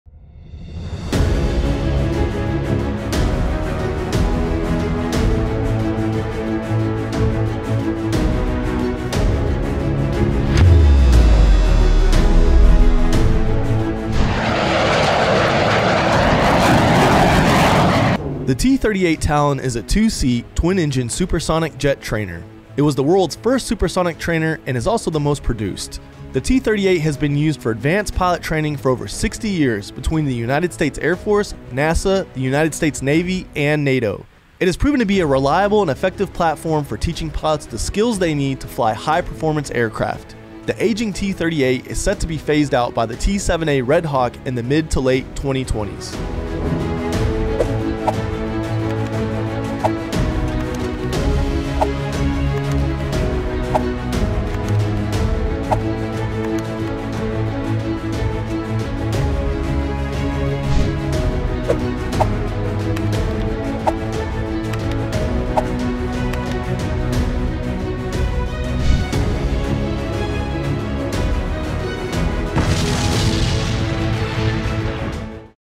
T38 Heritage Display
The T-38 Talon is a twin-engine, high-altitude, supersonic jet trainer used in a variety of roles because of its design, economy of operations, ease of maintenance, high performance and exceptional safety record.